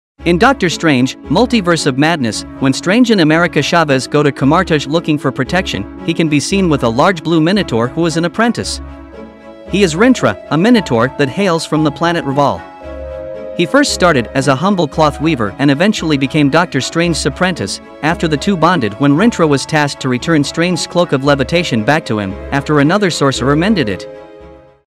Did you notice in Dr. Strange: Multiverse of Madness..?
In Dr. Strange: Multiverse of Madness, when Strange and America Chavez go to Kamar-Taj looking for protection, he can be seen with a large blue Minotaur who is an apprentice. He is Rintrah, a Minotaur that hails from the planet R'Vaal. He first started as a humble cloth weaver and eventually became Doctor Strange's apprentice after the two bonded when Rintrah was tasked to return Strange's cloak of levitation back to him after another sorcerer mended it.